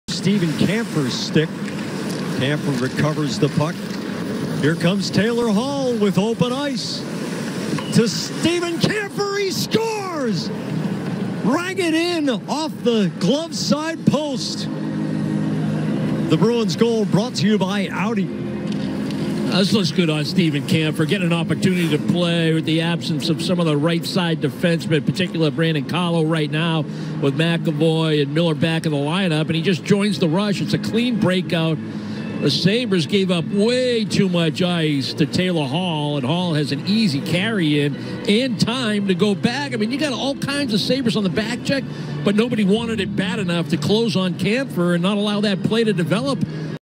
4/23/21 Steven Kampfer Opens The Scoring In Buffalo
The Bruins get out to the early lead as Steven Kampfer lets one rip from the slot beating Ukko-Pekka Luukkonen for the opening tally of the game.